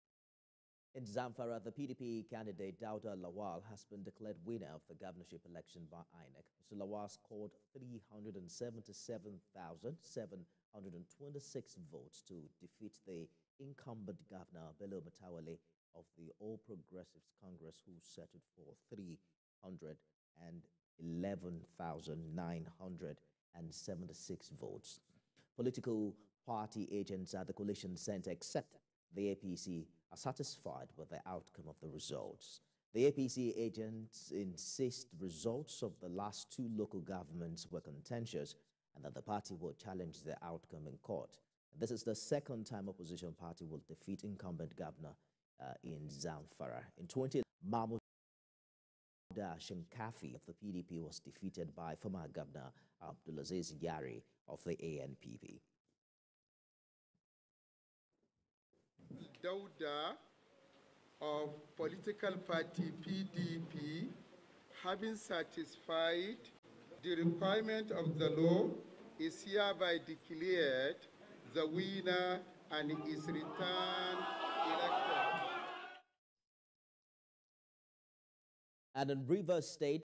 PDP's Dauda Lawal Unseats Gov. Matawalle in Zamfara State
Political party agents at the Collation Centre except the APC are satisfied with the outcome of the results.

The APC Agents insist results of the last two local governments are contentious and the party will challenge the outcome in court.

This is the second time opposition party would defeat incumbent Governor in Zamfara state. In 2011, Former Governor Mamuda Shinkafi of the PDP was defeated by Former Governor Abdulazeez Yari of the ANPP.